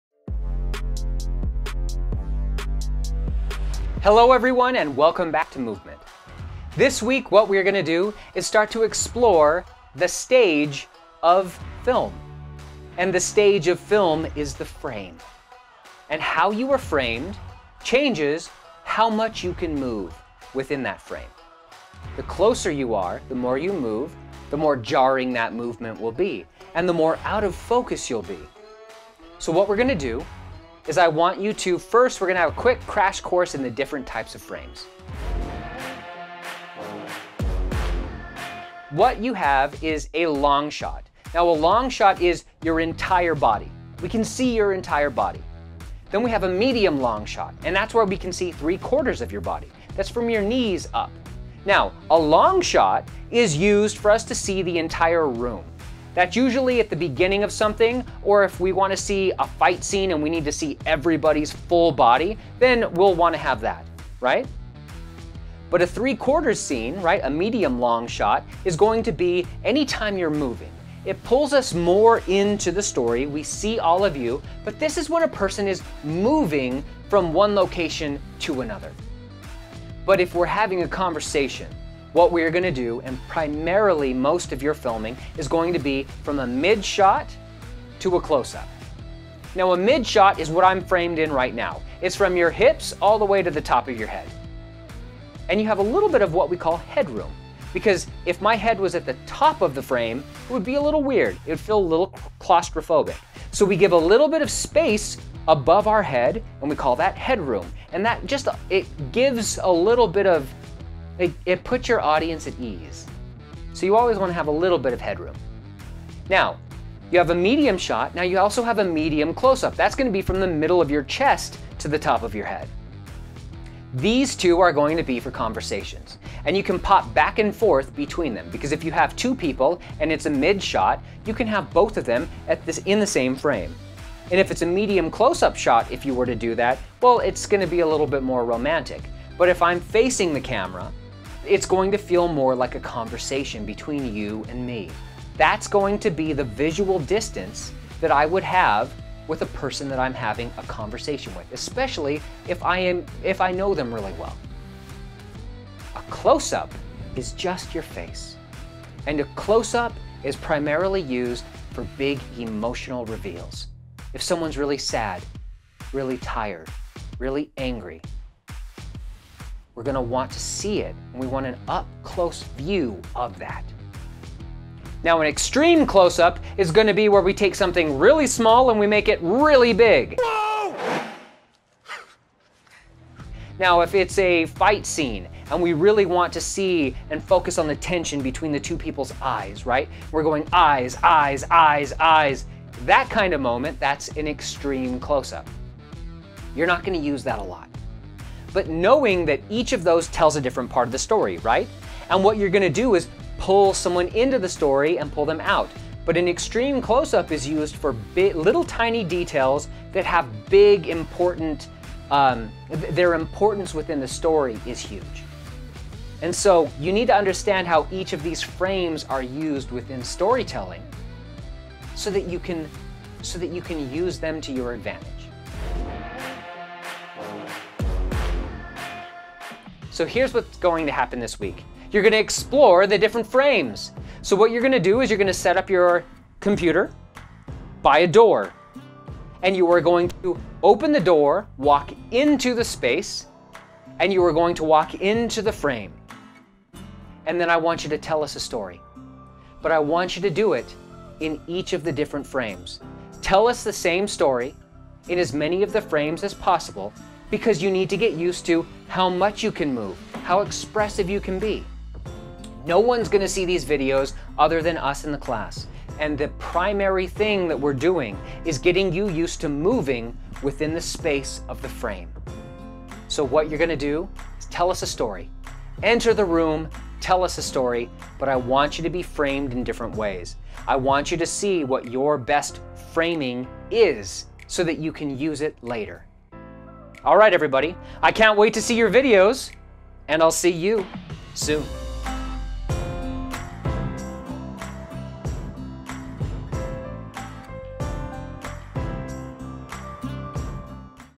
Types of Frames in Film
The stage of film is the frame and each frame is a tool with specific uses.  In this video we go over each type of frame from and Long Shot to an Extreme Close Up and how it is used in storytelling.